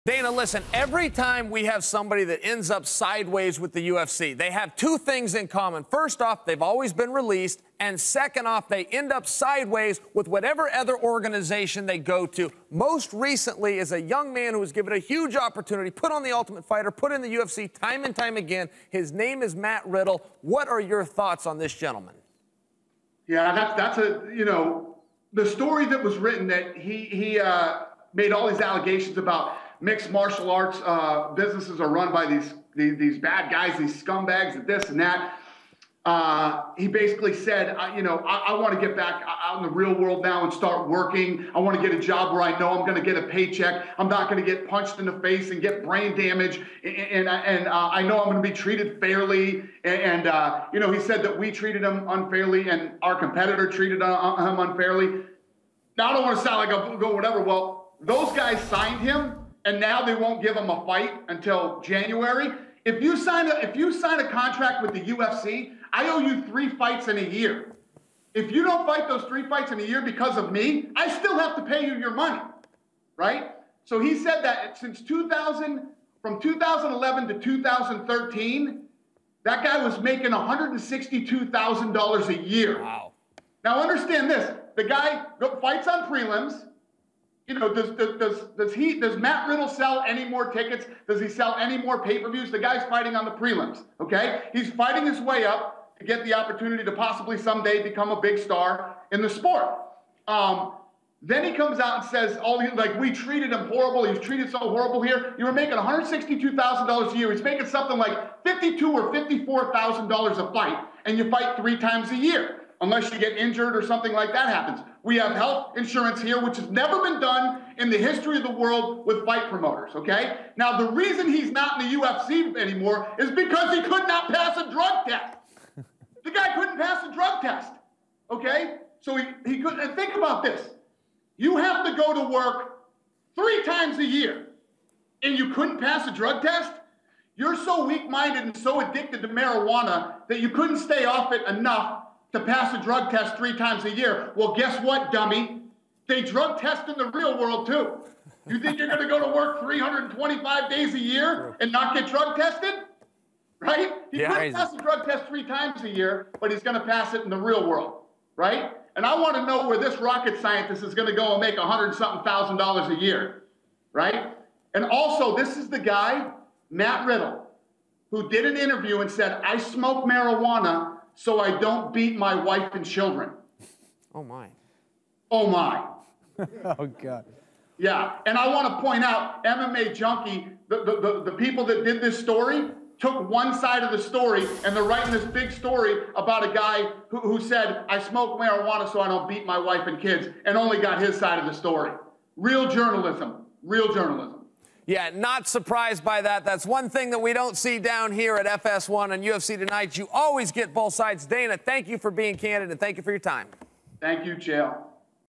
Dana White rips Matthew Riddle
Dana White responds to Matthew Riddle's criticism of the UFC and their inability to get him a fight.